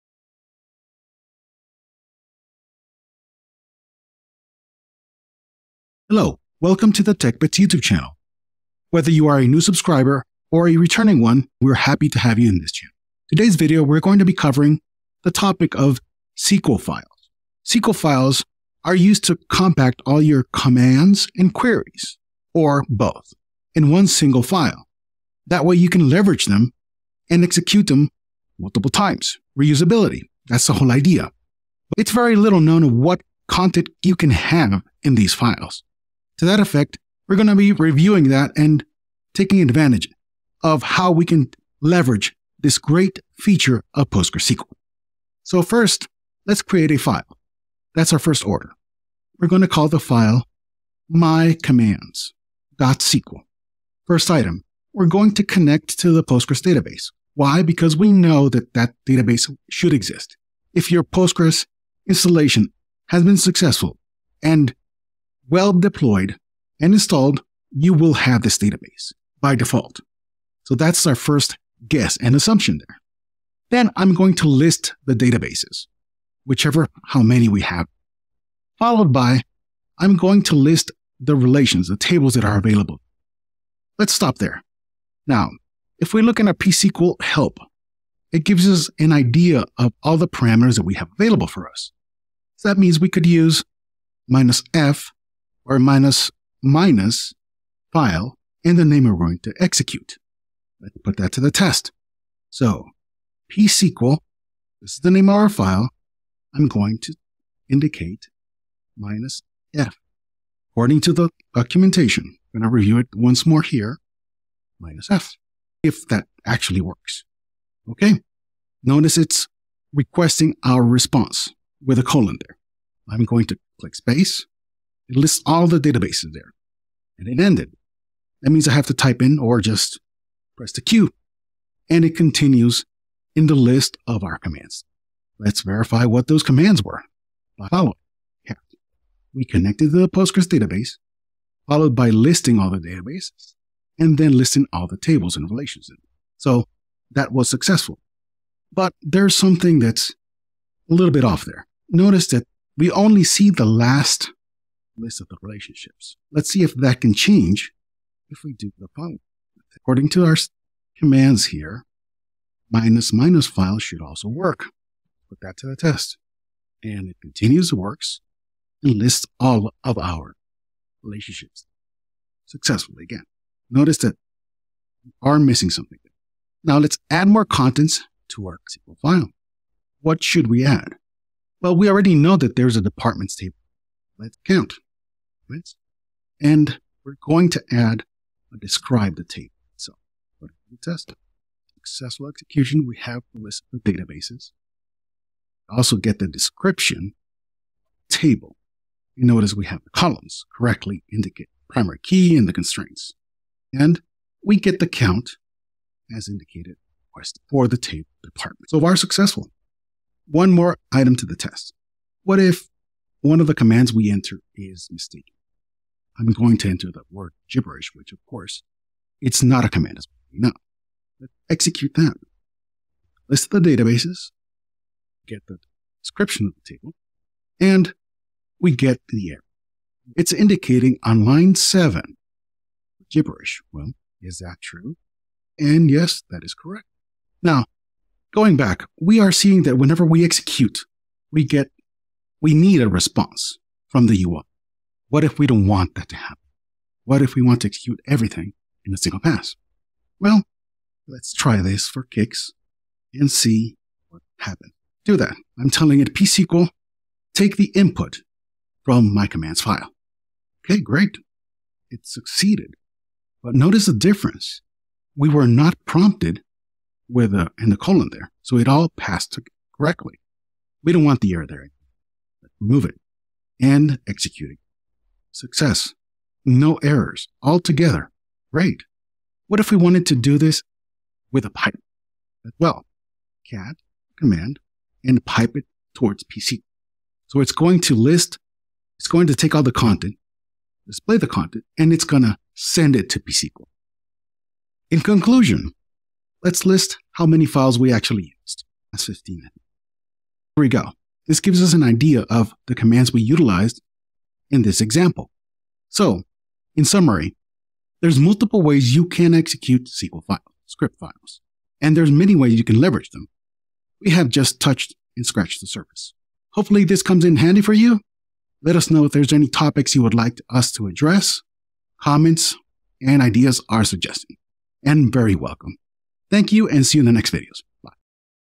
Can a Text File Really Work Data Magic? psqlSecrets Revealed! || Tech Bits
🤔🪄 Can a Text File Really Work Data Magic? psqlSecrets Revealed! ✨

🔍 Exploring psql Scripts

Tired of repetitive SQL tasks draining your data mojo?

‍♀️ Well, brace yourselves, wizards & witches, because .sql files are your potion to data mastery!

In my latest video, "Can a Text File Execute Data Magic? Exploring psql Scripts," we explore the hidden powers of these unassuming files.

Ready to level up your SQL game and unleash your inner data sorcerer?

Share your favorite data automation tips in the comments!  Let's brew up some data magic together! ‍♀️

database,dataanalysis,datascience,sql,automationmagic,watchnow,postgres,postgresql,databases,relational,relational databases for beginners,relational database

▬▬▬▬▬▬ ⏱ Timestamps ⏱ ▬▬▬▬▬▬
00:00 Intro
04:54 Conclusion
05:27 Outro